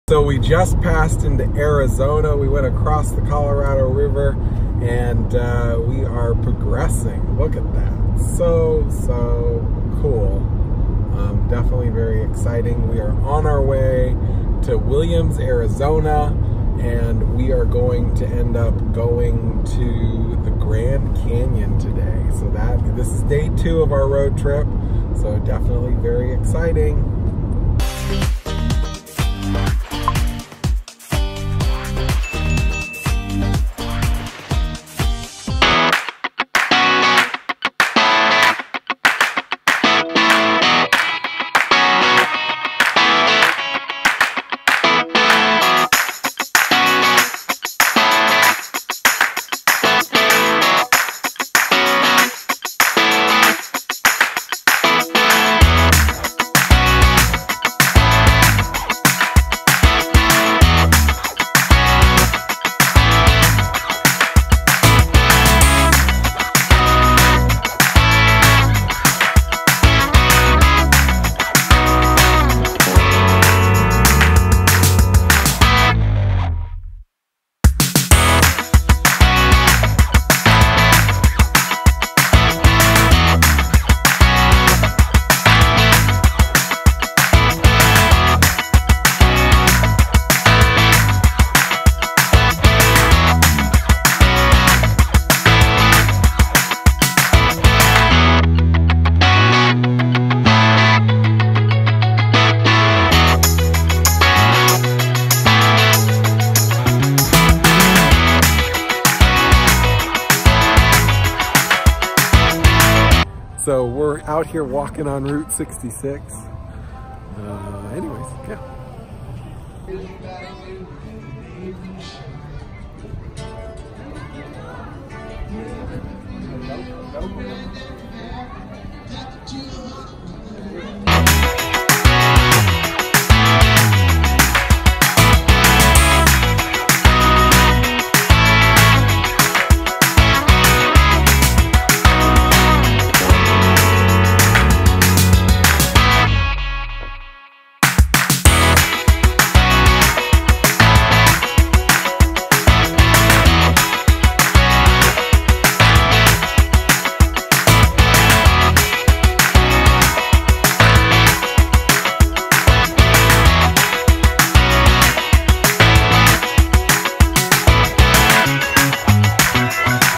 Road Trip Drive Day 2 - Interstate 40 California Barstow to William's Arizona
Checkout Day 2 of our drive. On this drive we went from Barstow to William's Arizona along Interstate 40.